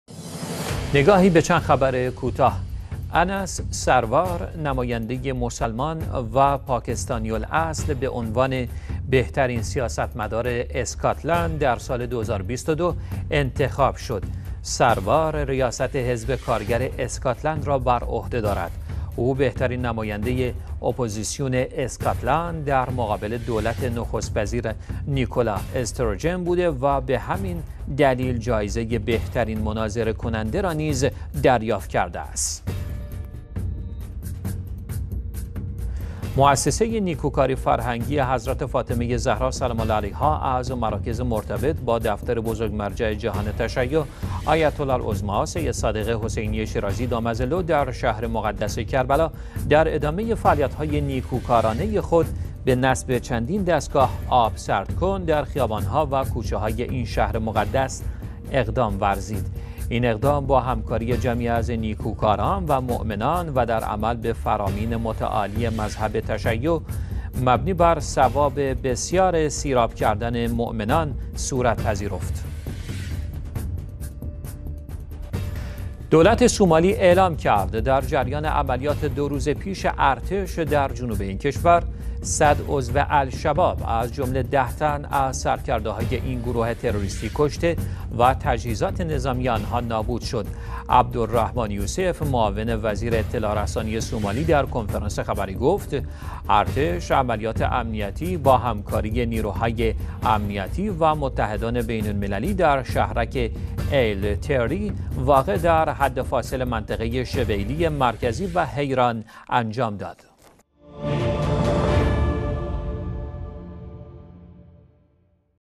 نگاهی به چند خبر کوتاه: دوشنبه 7 آذر ماه 1401
1-) انتخاب نماینده مسلمان به عنوان بهترین سیاستمدار اسکاتلند در سال ۲۰۲۲
2-) نصب چندین دستگاه آب سردکن در خیابان های شهر مقدس کربلا توسط مؤسسه حضرت فاطمه زهرا سلام الله علیها
3-) هلاکت ۱۰۰ سنی تندروی الشباب در عملیات ارتش سومالی